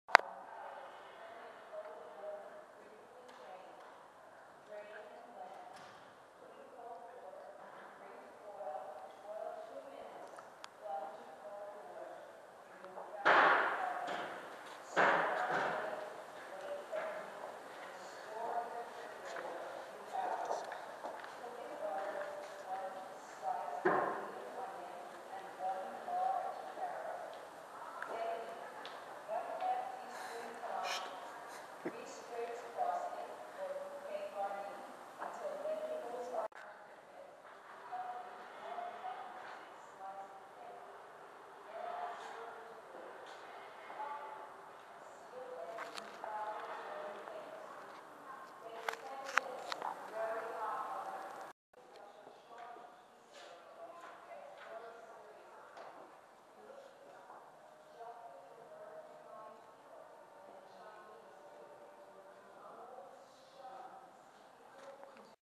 herman de vries - The Temporary Stedelijk 2
As of March 3, the Stedelijk Museum continues its temporary program with Temporary Stedelijk 2, which focuses on the renowned collection of modern and contemporary art and design.
A number of gallery spaces are devoted to individual artists and designers. Works by Carl Andre, Lothar Baumgarten, Barbara Bloom,&nbsp;herman de vries,&nbsp;will be on view. herman has his own space presenting publications and editions from the colelction of the Stedelijk Museum.